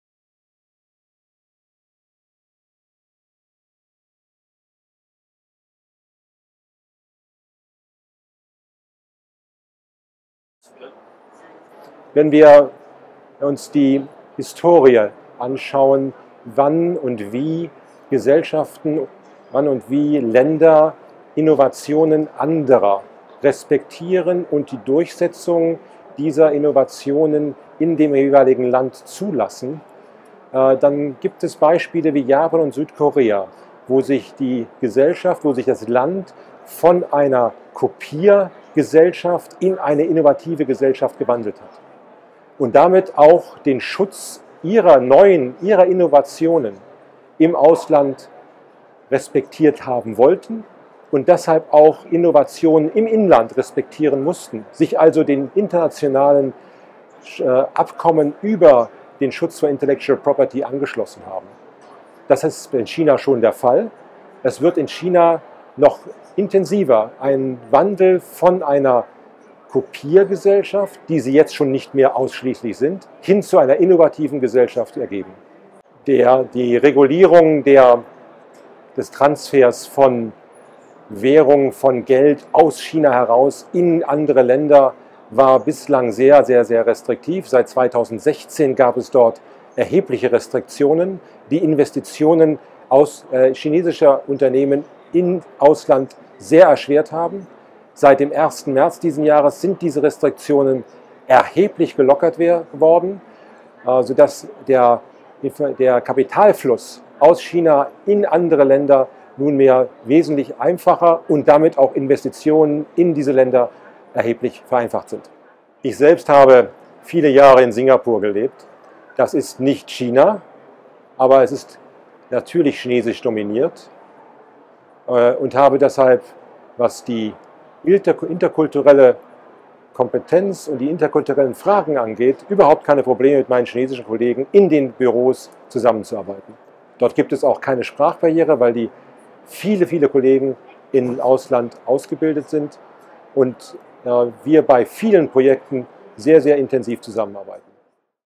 Finance Day 2018: Peter Homberg, Partner Dentons Europe LLP
…über Innovationen in Japan und Südkorea sowie Investitionen aus China.

Das Interview fand im Rahmen des Finance Day am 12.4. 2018 auf der analytica  statt.